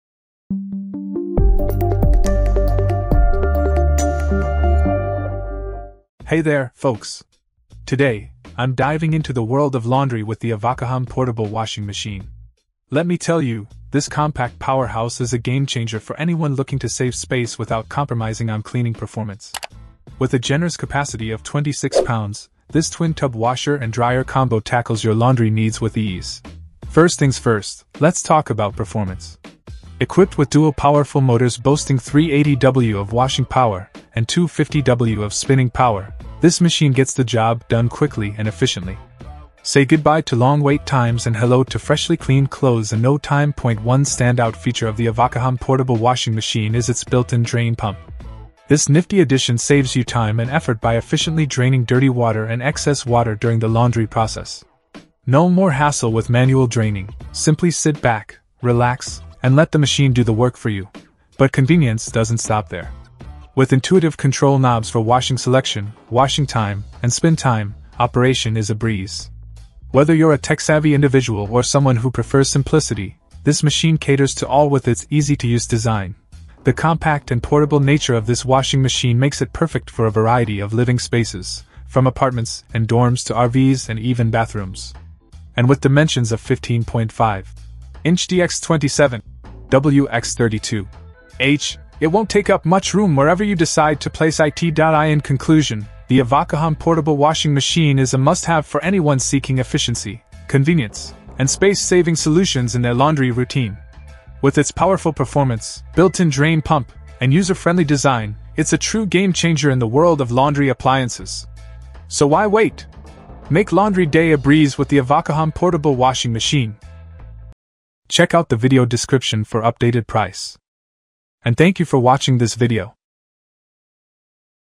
reviewAvocahom Portable Washing Machine: The Ultimate Laundry Companion
Avocahom Portable Washing Machine: The Ultimate Laundry Companion

0:00 Intro
0:06 Review

Things that we mentioned in this video:
Portable Washing Machine, 26/28Lbs : B0C4X76TQJ